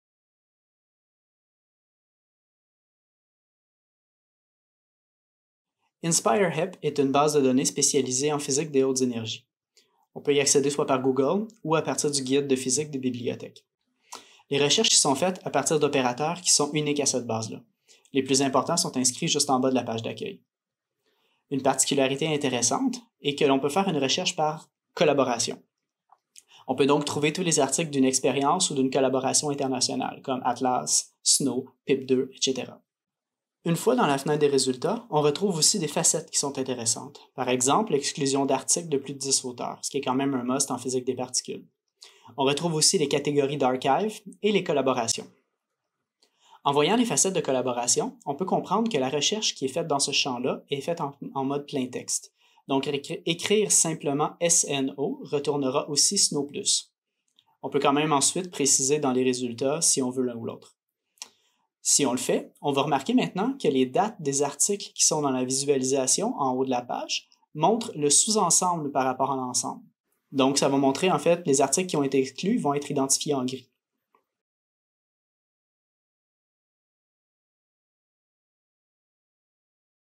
Caractéristiques et fonctionnement d'INSPIRE-HEP
Cette vidéo décrit les caractéristiques intéressantes de la base de données INSPIRE-HEP, spécialisée en physique des hautes énergies.